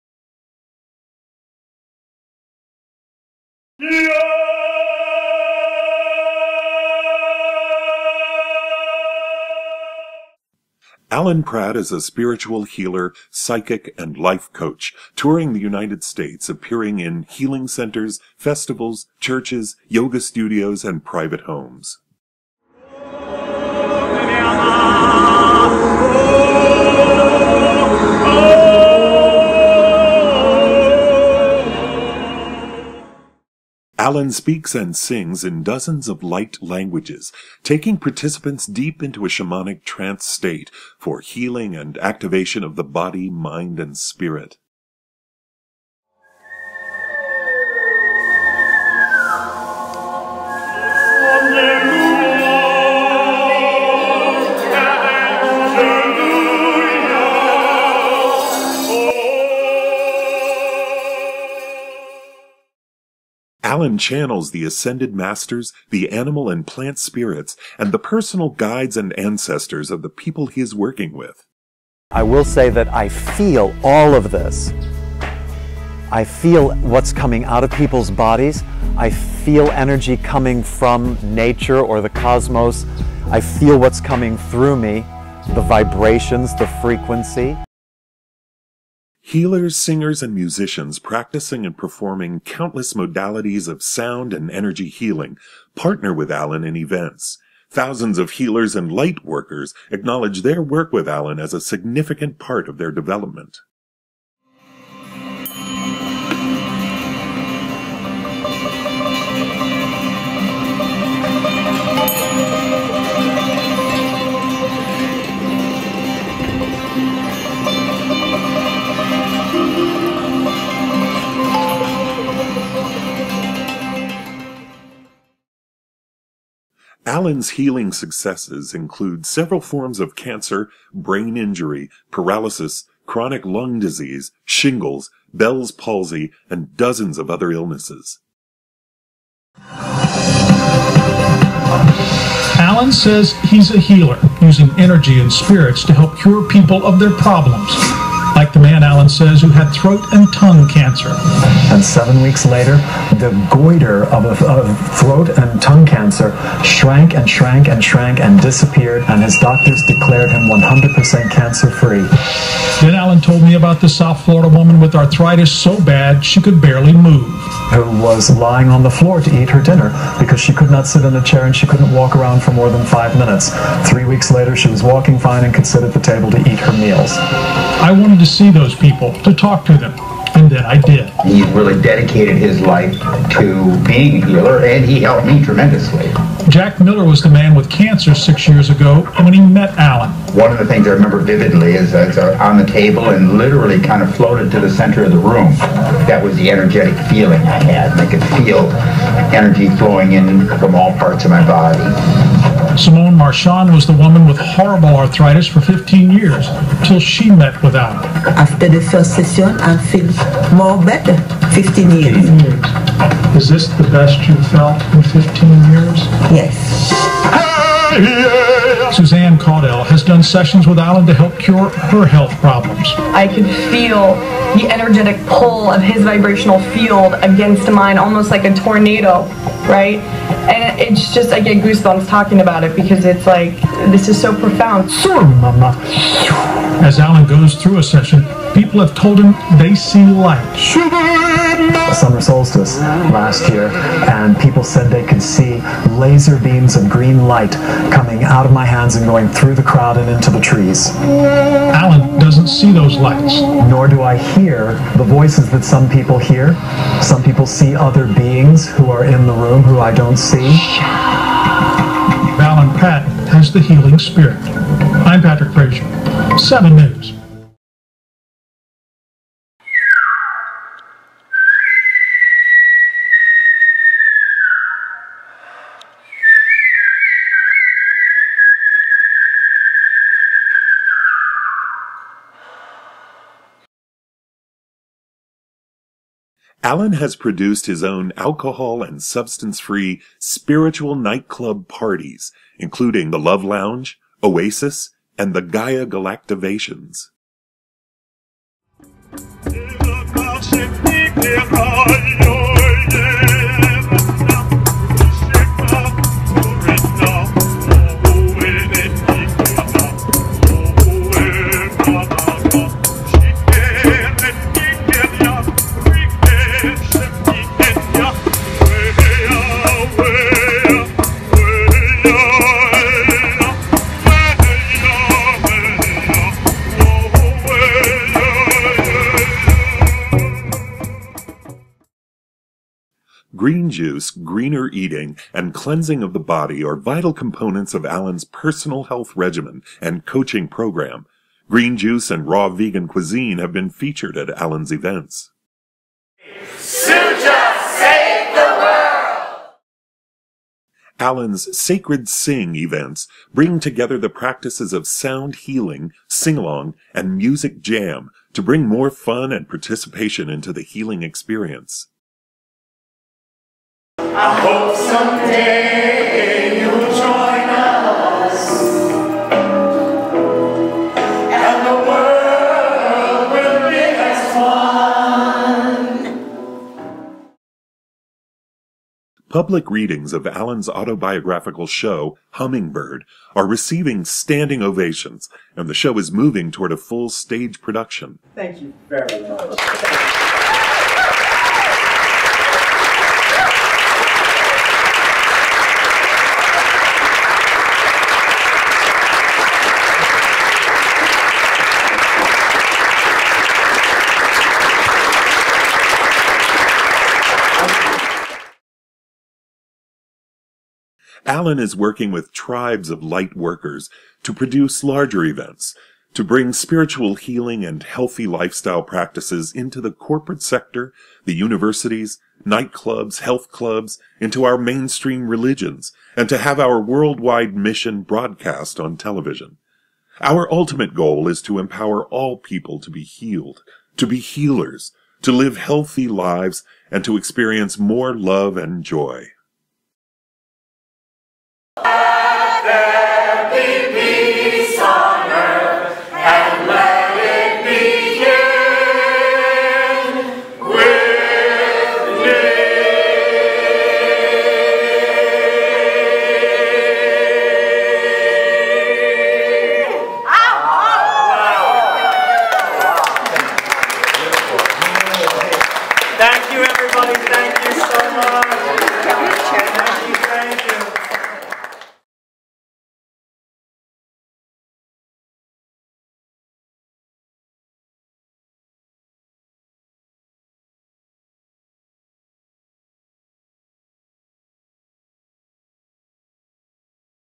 Alan Pratt Promotional Video
This promo clip was shot in 2015.

Alan Pratt has toured the U.S. since 2009, conducting Sound Healing Ceremonies & Concerts, facilitating Psychic & Life Coaching Circles, and presenting his own original spiritual theater pieces. Alan has worked with hundreds of healers and musicians, practicing countless healing modalities. Thousands of light workers acknowledge Alan for activating their abilities to higher levels.

In ceremony, Alan sings & speaks in many 'celestial languages', taking participants deep into a shamanic trance state, where we connect with spirit guides, purge old trauma, heal disease & activate our dormant DNA.

Here are links to 5 Facebook posts that illuminate the range of Alan's work:
Successful Healings in One Session or Event -- 23 Cases
Brain Injury, Paralysis, Spinal Injury -- and the Power of Remote Healing